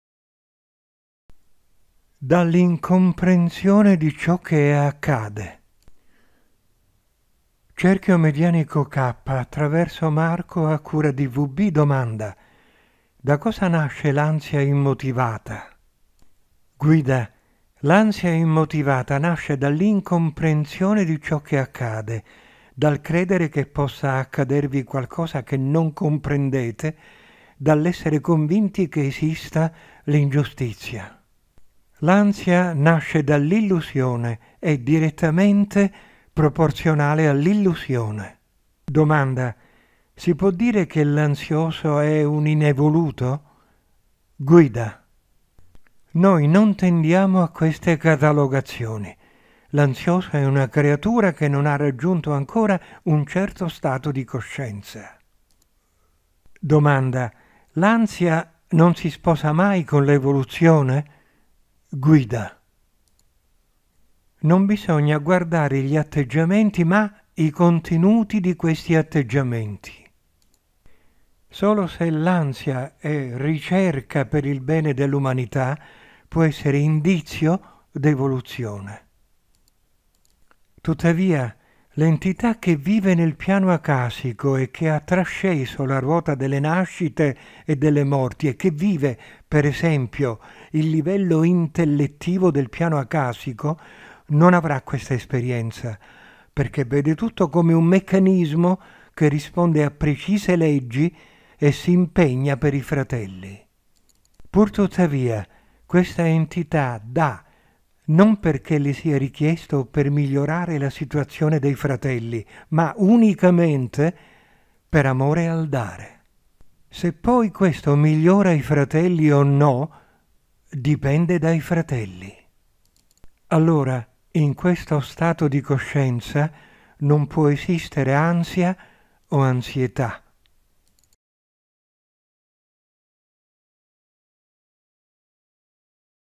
Evolvenza Dall'incomprensione di ciò che accade
The love that unites two creatures is an indissoluble bond which, when it is powerful, is perpetuated through the various incarnations. Love constitutes consciousness and is the goal towards which the cycle of rebirths tends.